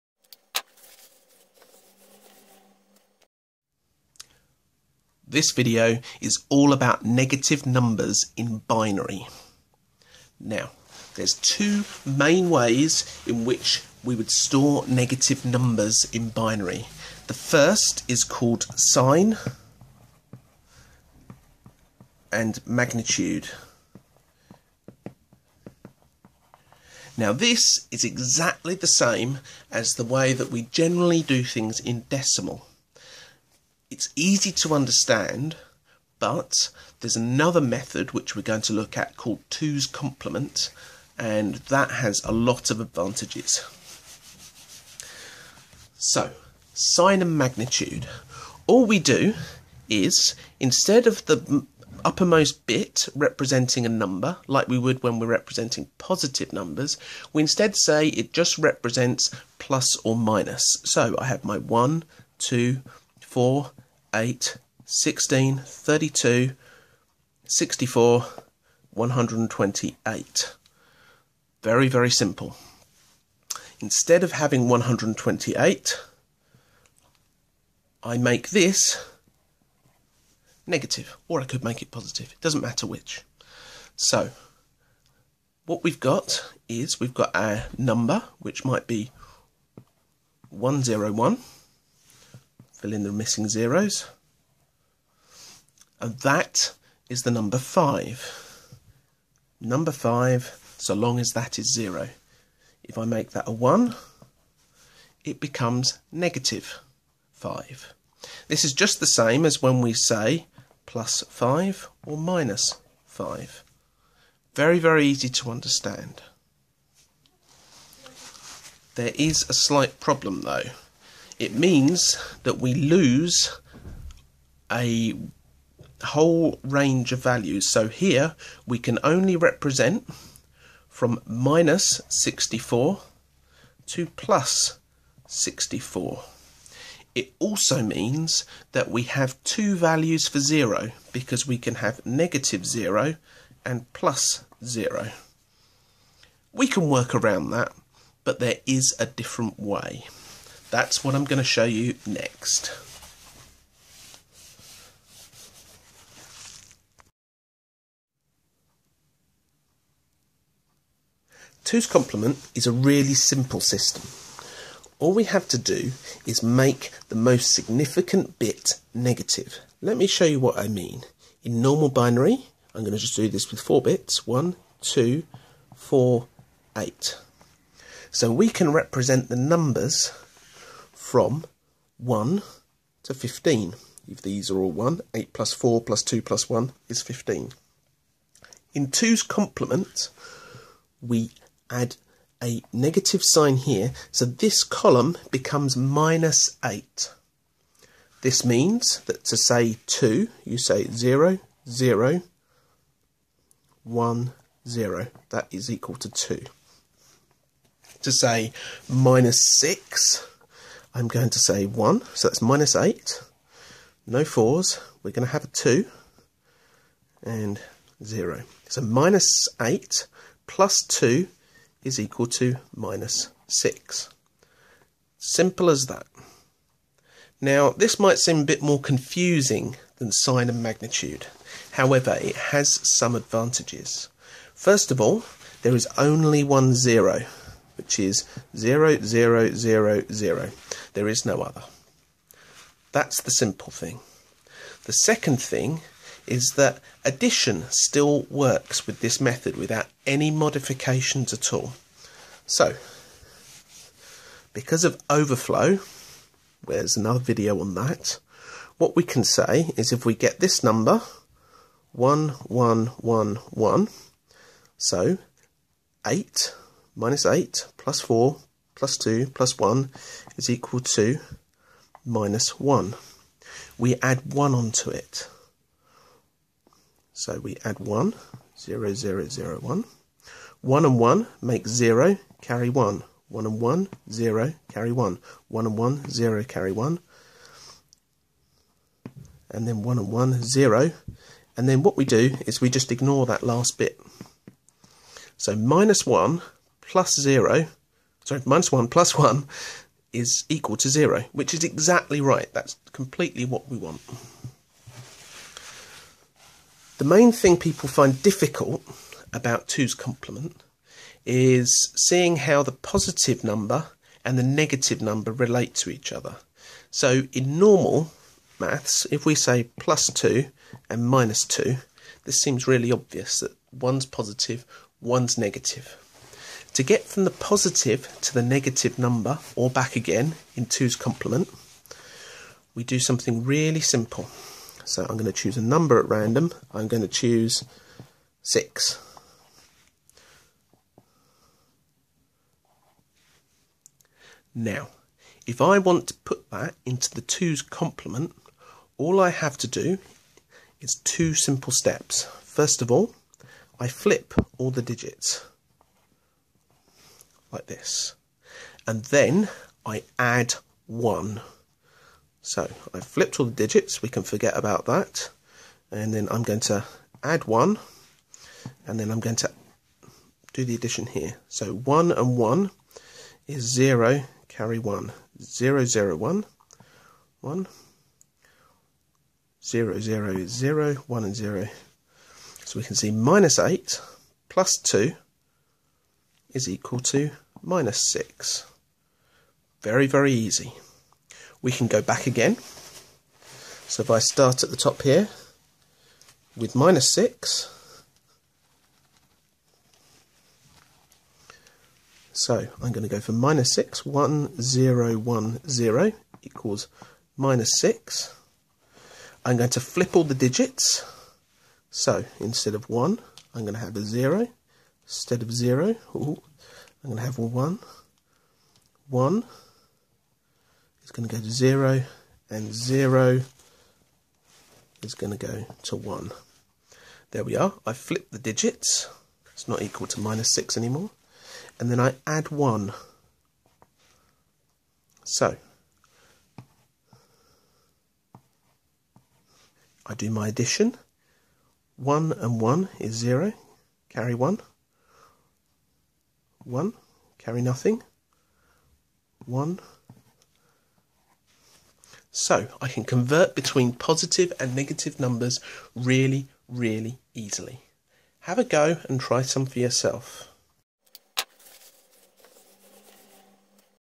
Negative Numbers in Binary
How to use two's complement or sign and magnitude to represent binary numbers.